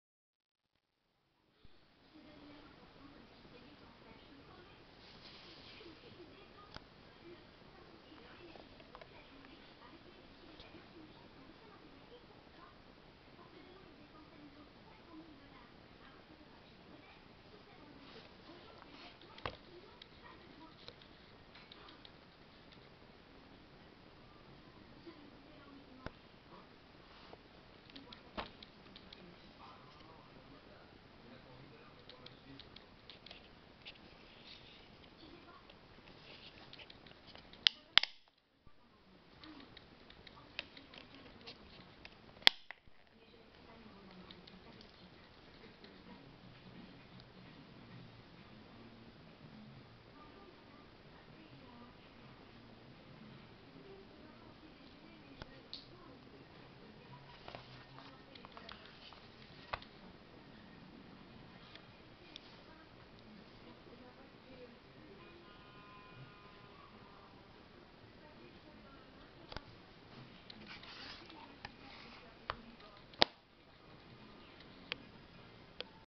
LA BELLE ET LA BETE STEELBOOK BLURAY AVEC ARTWORK INTERNE MODOFIE [FR]
steelbook fnac sans artwork interne a l'origine.